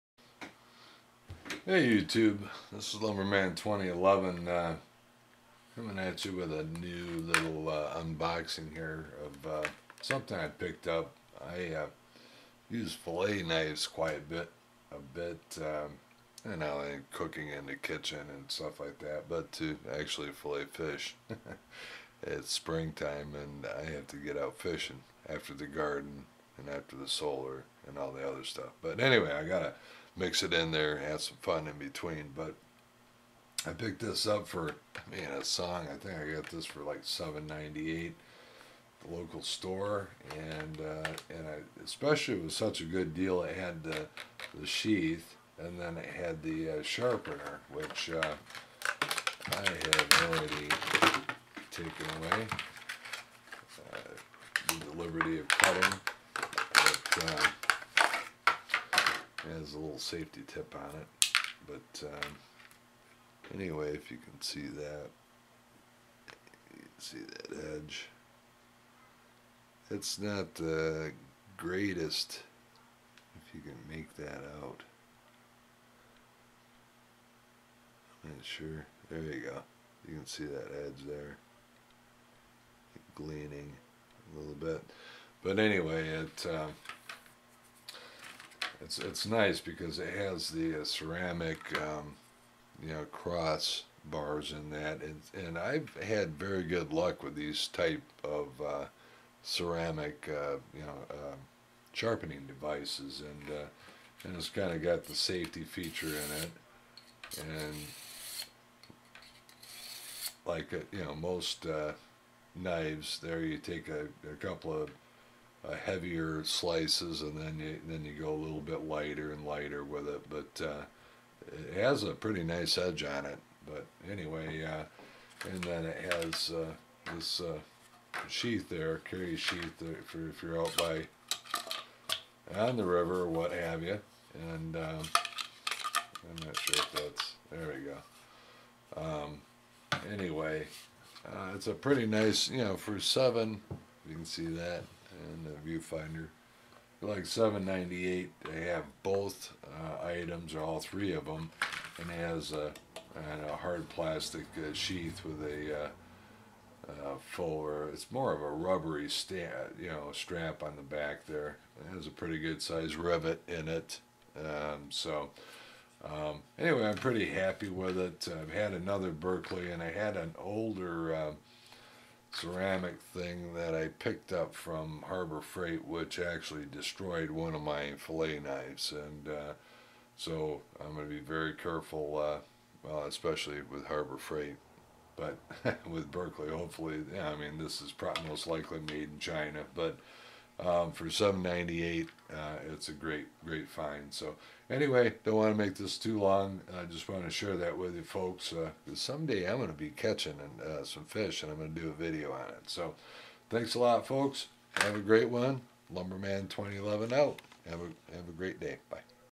Berkley Filet Knife, Sheath and Ceramic sharpener- A Great Deal!
I folks, just picked up this great deal On a Berkley filet Knife with Sheath and Ceramic Sharpener for $7.98 or $8.98 at a local store. This combo is a great deal. Hope to use it on some fish soon as spring is here and they will be on the beds soon!